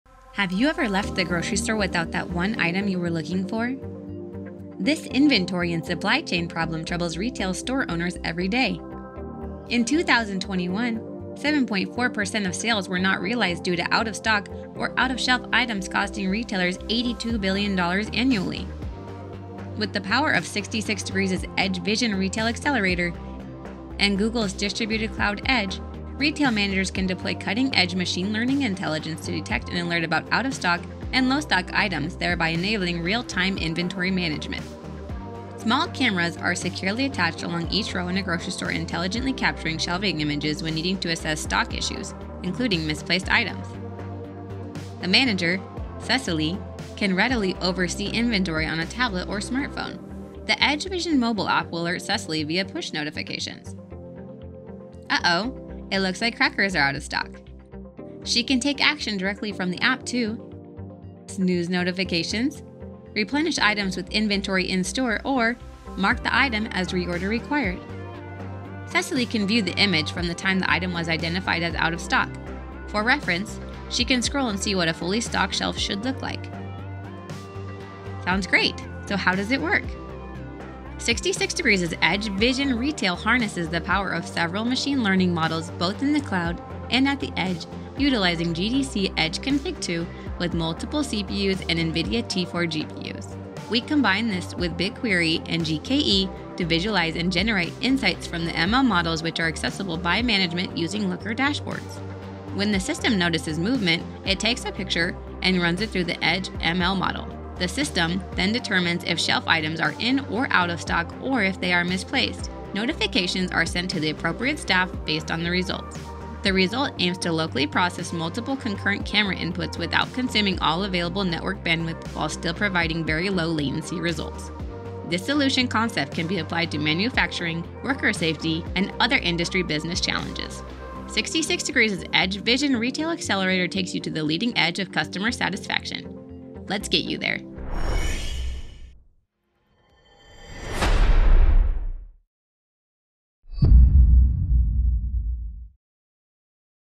[DEPRECATED] Real time inventory management with 66degrees and Google Distributed Cloud Edge
Learn how 66degrees is enabling real time inventory management to improve operational efficiency on Google Distributed Cloud Edge. Continue learning about GDCE